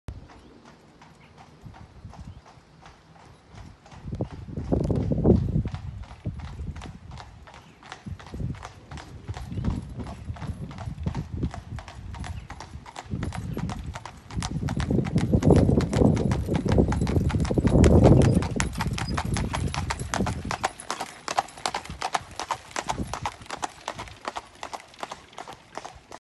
Wedding Carriage Arrives
The wedding carriage arrives to collect the Bride and the Bride's father.